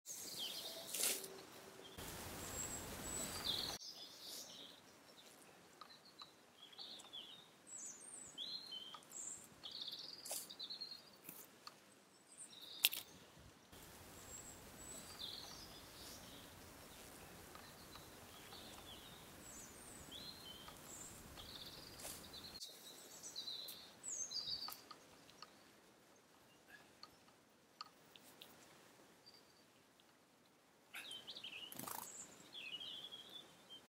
Robin finds a juicy Earthworm to eat. Allotment Digging.
British robins are fearless birds and will often follow us at the allotment while we dig, hoping to catch a tasty worm.

Robins are ground feeders, hopping around lawns and flowerbeds, and beneath trees and shrubs, where their large eyes enable them to find their invertebrate food even in the lowest light.